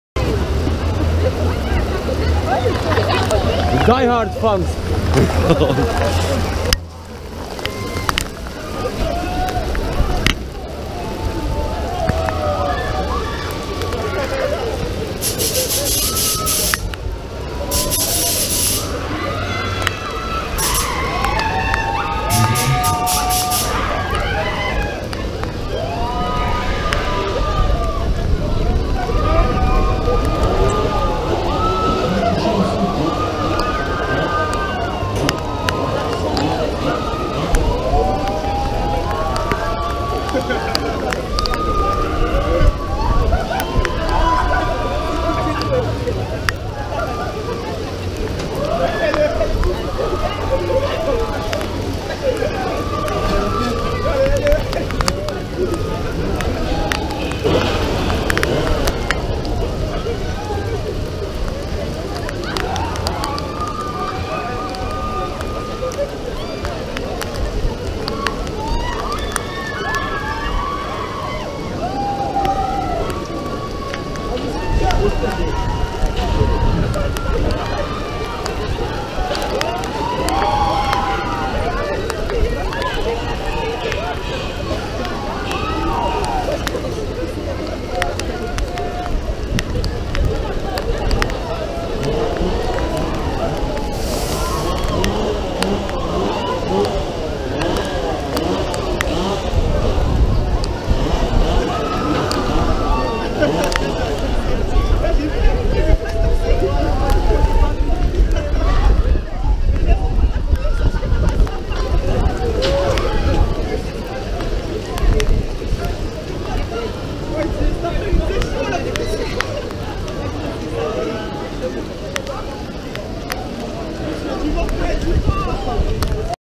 "Inversion 360" in the rain (Foire du Trône, Paris, France) 2016
Builder: Technical Park
Type: Streetfighter Revolution
Owner: Baptiste